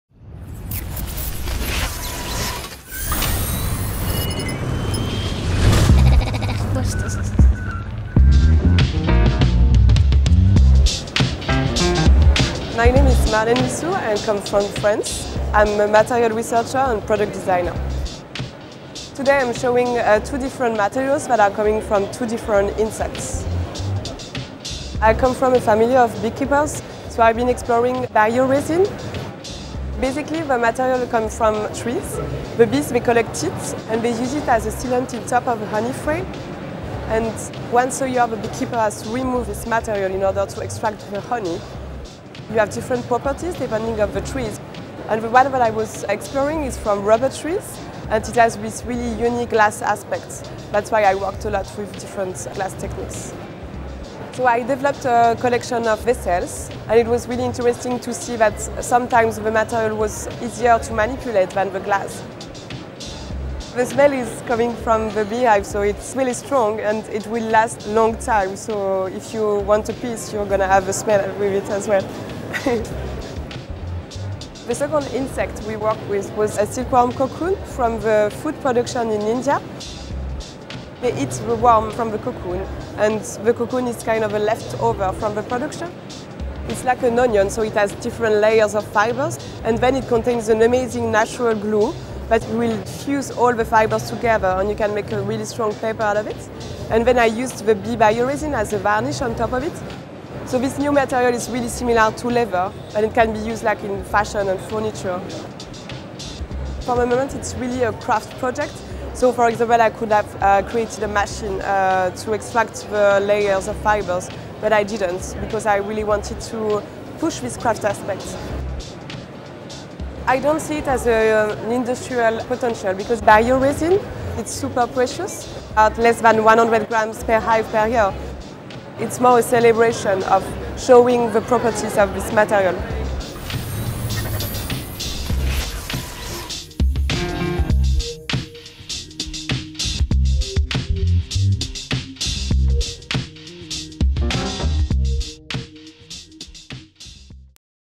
Marlène Huissoud uses insect by-products to develop new materials
French designer Marlène Huissoud explains how she produced a range of "glass" and "leather" objects using resin collected from beehives and silkworm cocoons in this movie from Eindhoven.

Huissoud's project From Insects aims to showcase the "unique" and "precious" materials that can be created from insect by-products.